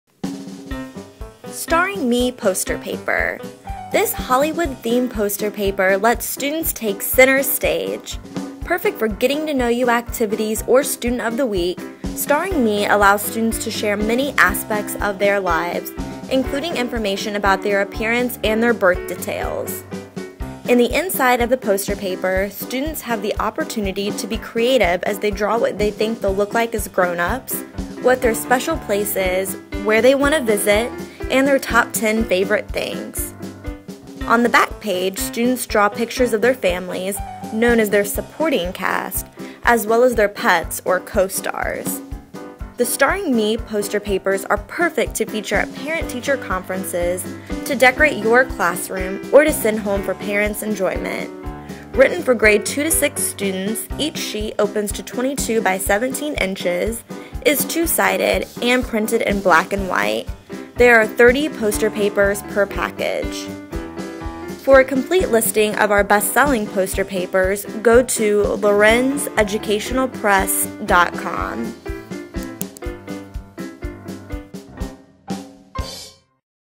Starring Me Poster Paper
This Hollywood-themed poster paper allows each student to be center stage in this innovative, interactive product. Each two-sided, black and white poster opens to 22" x 17"; 30 posters per package.